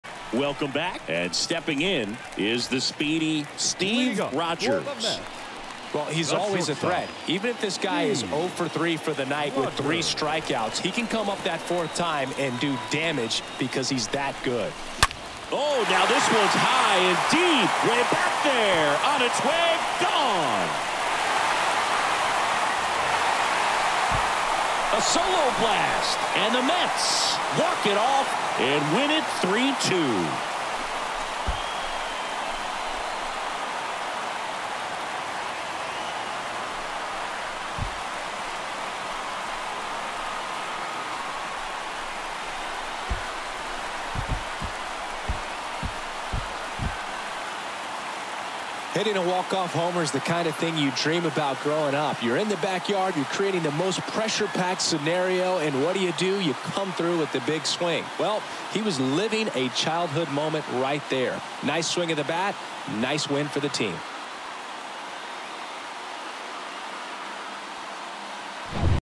9th Walk-off HR
The elite shortstop of the Mets secured his 9th Walk-off HR